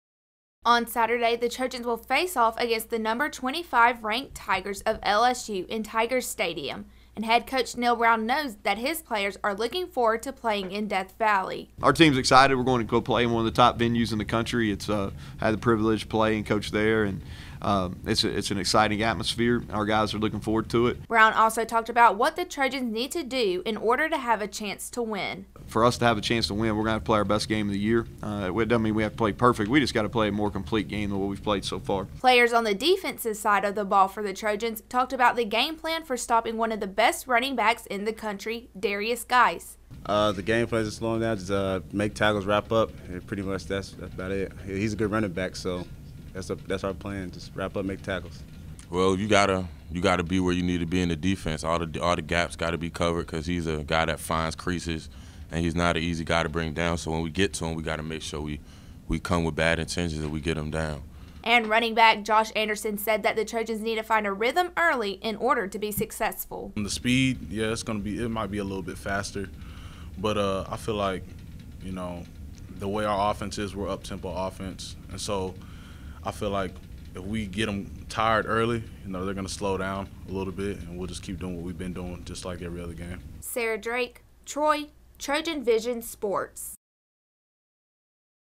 TROY Football vs LSU - TROY TrojanVision News
The Trojan football team is coming of a thrilling 22-17 victory over the Akron Zips and this week TROY will make a trip to Death Valley riding a three game winning streak despite not putting together a complete effort in all three phases of the game. TROY TrojanVision News’ Sarah Drake gives us a preview of the Trojans upcoming contest versus the Tigers of LSU.